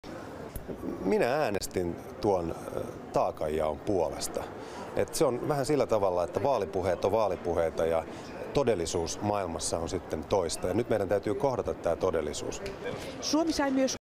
Petri Sarvamaa, taakanjako
Petri Sarvamaa Twitterissä:
"Vaalipuheet on vaalipuheita" -lausuntoni Kympin uutisissa viittaa tietenkin persujen vaalipuheisiin - ei omiini :)